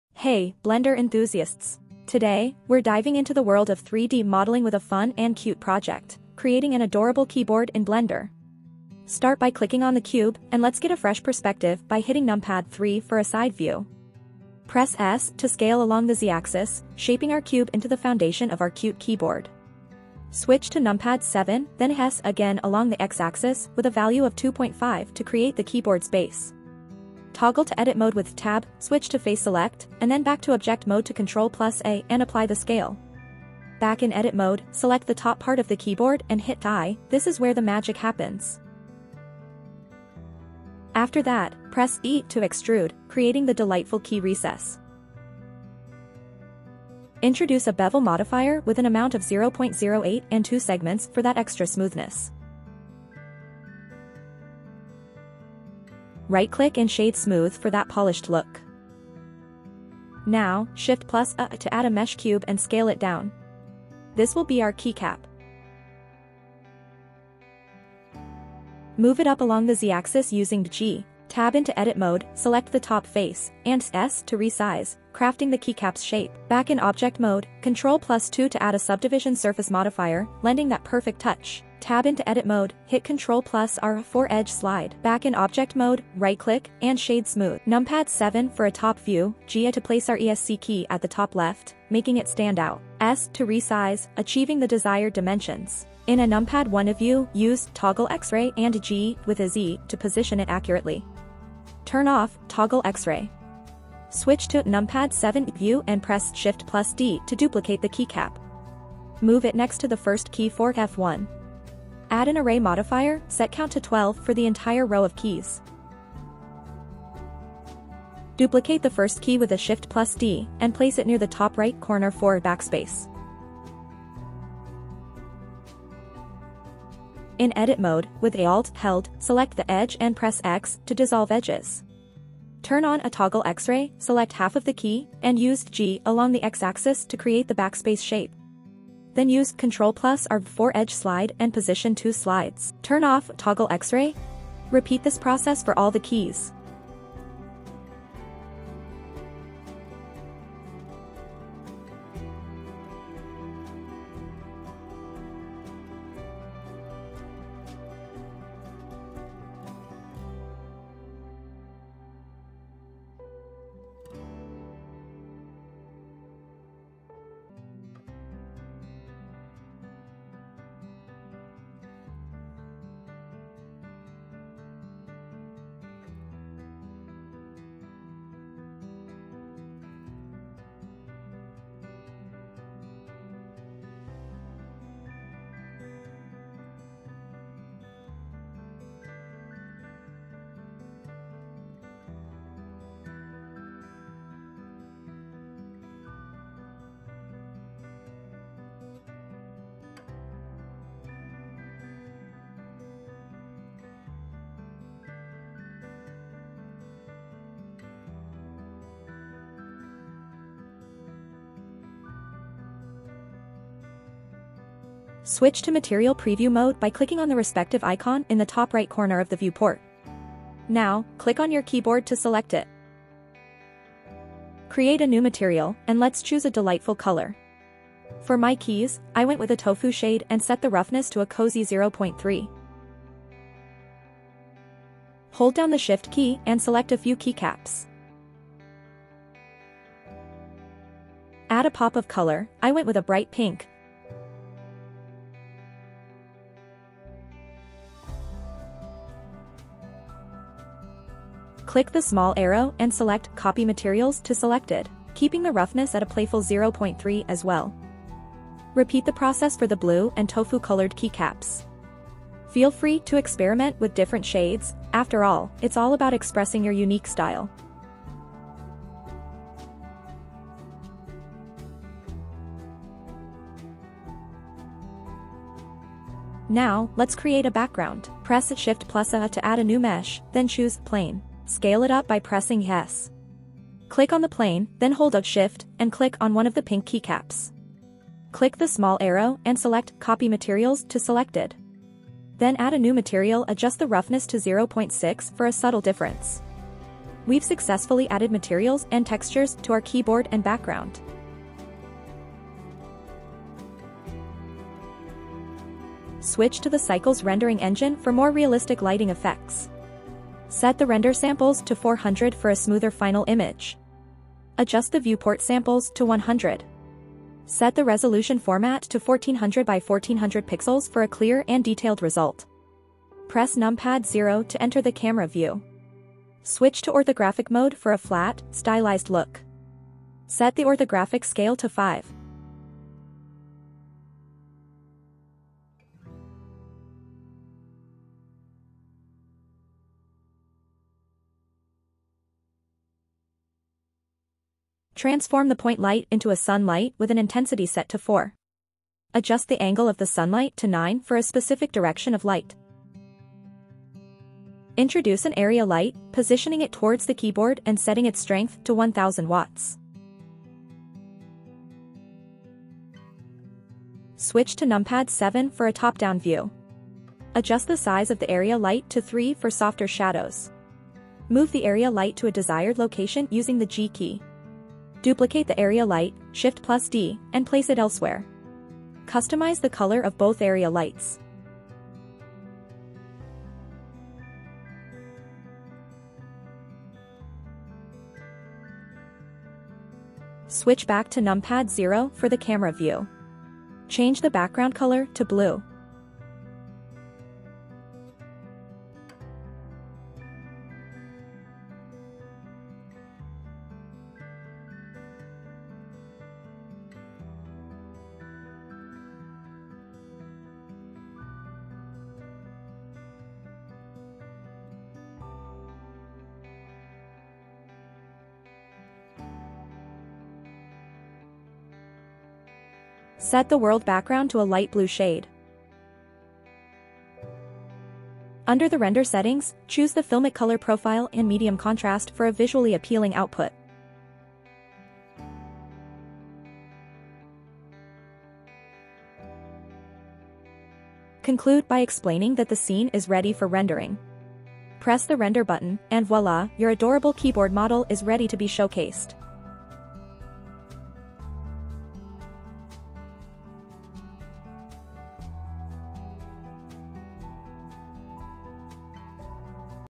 How to create 3D Cute Keyboard in Blender 3D
Welcome to my delightful Blender tutorial, where we'll embark on a journey to model an adorable keyboard! 🎨 In this tutorial designed for beginners, I'll guide you step by step through the creative process of crafting this charming and whimsical keyboard model.

Just like in the gingerbread man tutorial, I want to highlight that these are my creative solutions, and there's always room for your personal touch. Blender is a canvas for your imagination, and I encourage you to explore your own unique approaches to 3D modeling. As always, I value your feedback and welcome any suggestions to enhance the quality of my videos.

So, let's jump into the tutorial! Here's what our creative journey will look like:

00:00 Intro
A warm welcome to the tutorial and a brief overview of what we're about to create.
00:08 Modeling
Dive into the exciting world of 3D modeling as we bring our keyboard to life.
03:30 Materials
Explore the vibrant world of materials as we add delightful colors to our keyboard keys.
05:05 Lighting and Rendering
Illuminate our scene with creative lighting and bring our keyboard model to life in the final rendering.
07:35 Thanks for watching

I hope you find this tutorial not only informative but also a spark for your own creative endeavors. 😊

I'd also like to extend my apologies if the video experienced some lag during the render view towards the end. Rendering can be a bit demanding, especially on a potato notebook, making it challenging to showcase seamlessly. I appreciate your understanding, and remember, it's the creative process that counts the most! 🥔💻😊

Don't miss out on more Blender tutorials and 3D creations!

And a massive thank you for being part of this journey and watching the video! ❤️